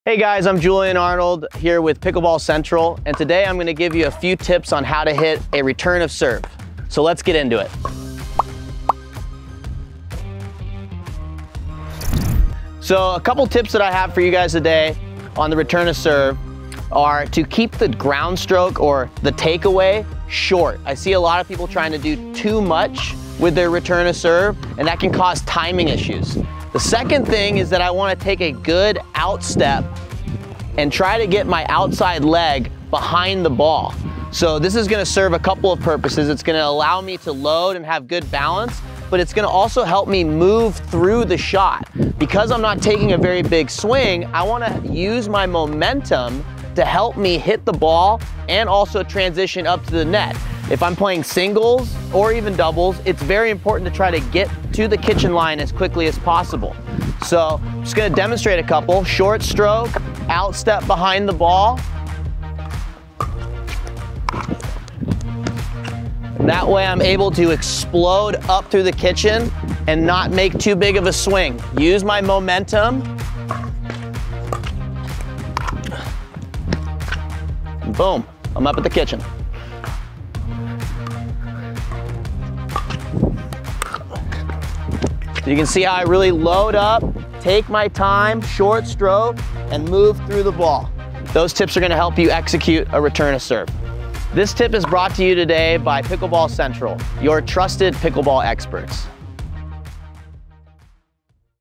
Return of Serve with Julian Arnold and Pickleball Central
Julian Arnold is a pro pickleball player and co-founder of Volair Pickleball. Check out this video where he gives his tips on how to master the return of serve in pickleball. Try it out and let us know if it helps you!